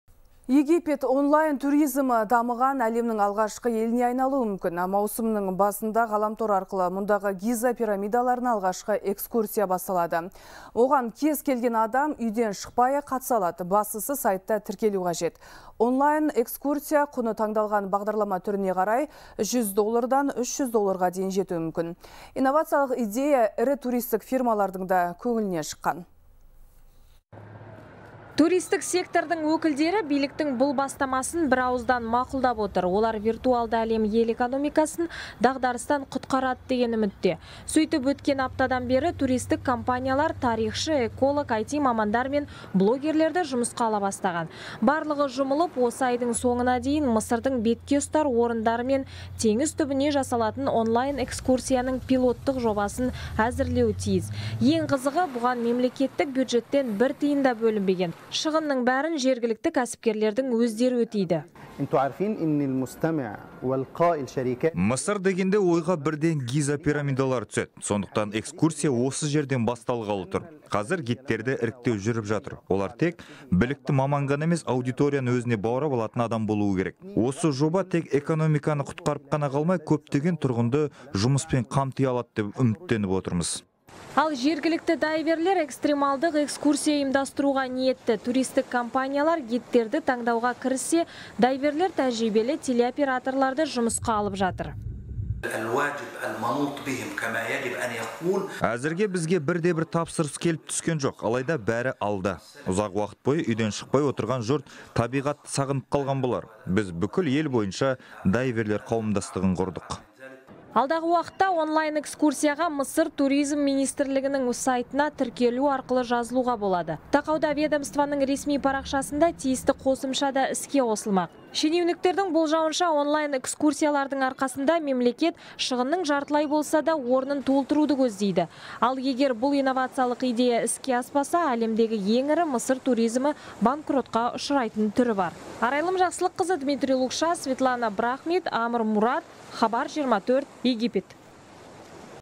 Египет онлайн туризмі дамыған әлемнің алғашқы еліне айналуы мүмкін
Маусымның басында ғаламтор арқылы мұндағы Гиза пирамидаларына алғашқы экскурсия басталады, деп хабарлайды «Хабар 24».